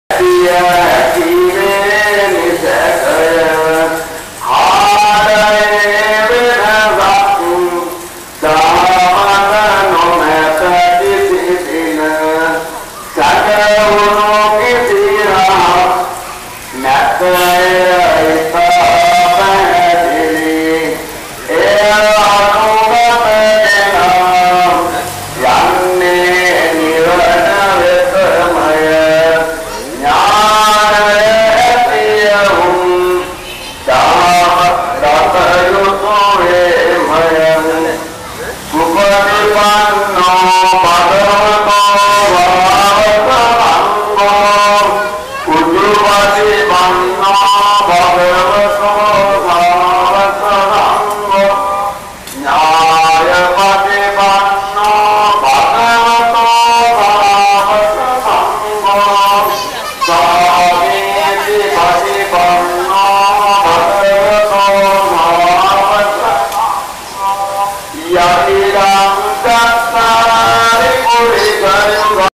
S0346R4-Colombo Gangaramaya.MOV
5/9/11 Hitting Gangaramaya Temple during prayers. Very interesting. Frequented by locals and very few tourists (if any).